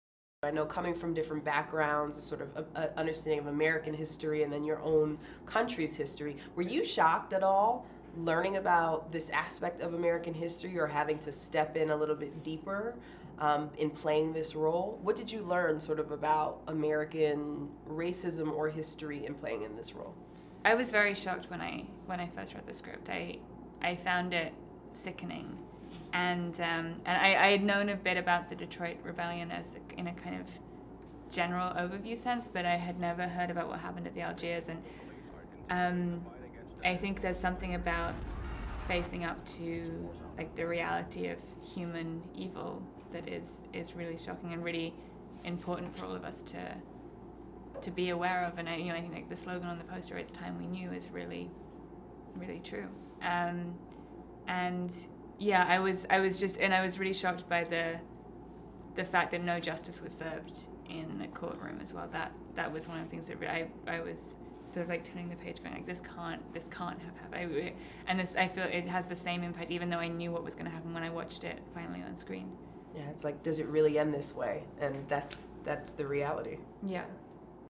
English Actress Hannah Murray was shocked by American racism in 'Detroit'
'Detroit' Actor Hannah Murray discusses researching the 1967 race riots for her role, and being shocked by her findings.